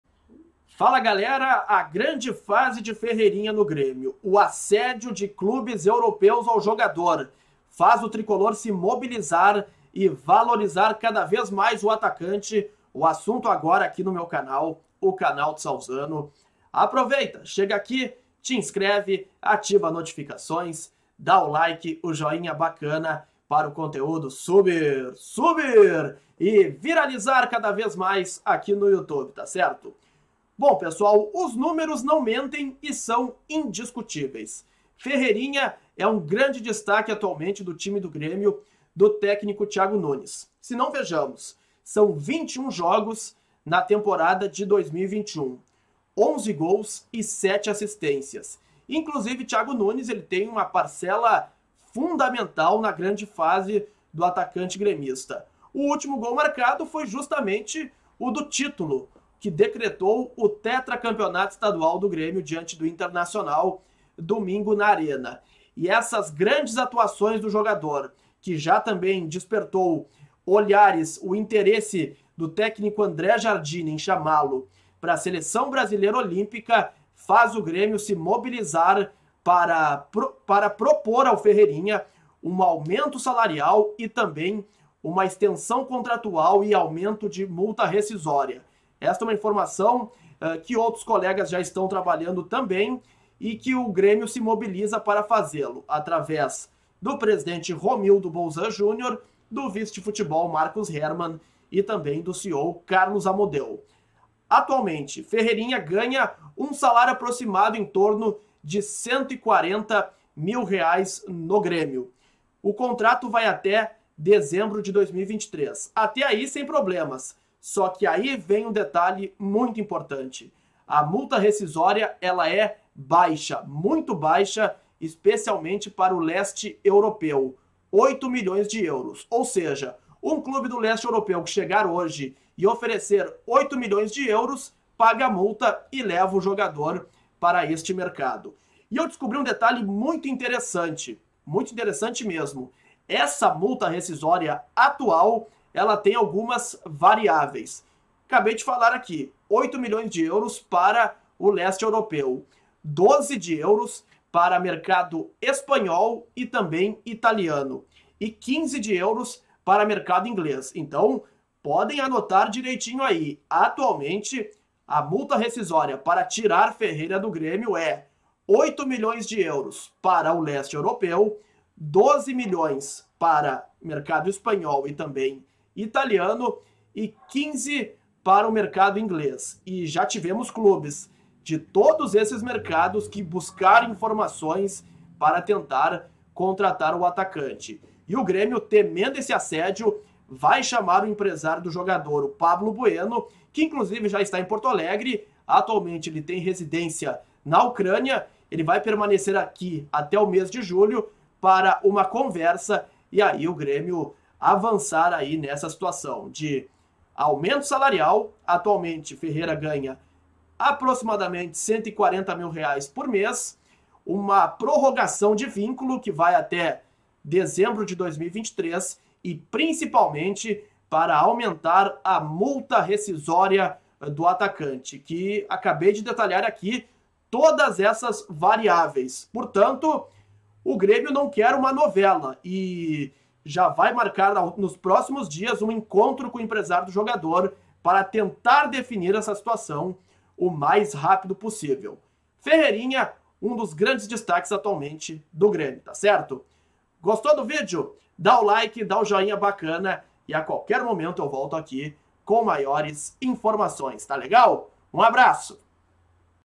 GRÊMIO QUER VALORIZAR AINDA MAIS ATACANTE FERREIRA.
Ciente do interesse de muitos clubes europeus, tricolor se antecipa e quer ampliar contrato do atacante, aumentar o salário e sua multa rescisória.